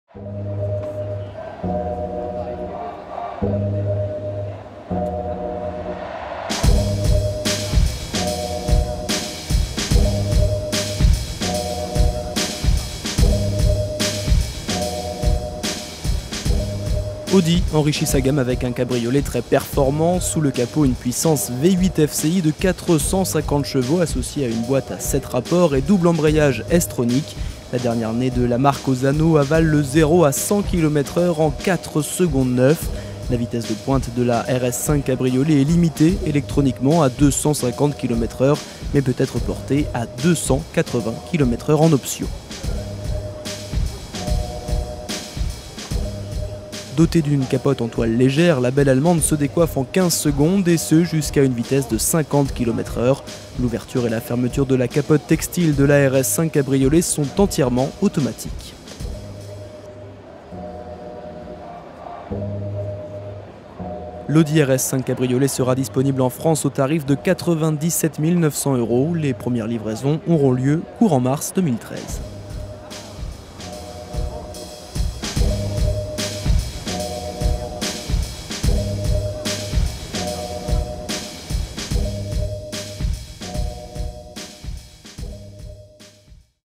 Audi RS5 Cabriolet au Mondial de l'Automobile 2012
Audi enrichit sa gamme avec un cabriolet très performant. Propulsée par un moteur 4.2 à injection directe de 450 ch associé à une boîte à 7 rapports et double embrayage S Tronis, l'Audi RS5 Cabriolet avale le 0 à 100 km/h en seulement 4,9 secondes. Découvrez-la en vidéo sur le stand Audi lors du Mondial de l'Automobile 2012.